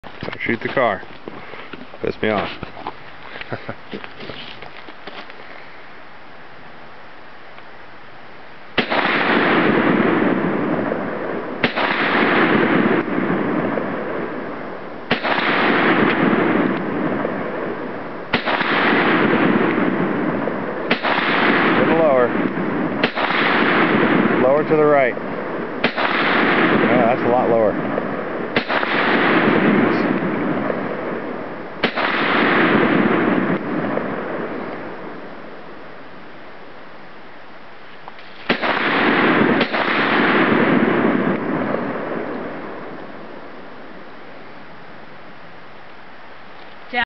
sks-pwns.AVI
Me shooting chinese (Norinco) SKS. 20 round clip.  Shooting Pumpkins at about 60 yards away.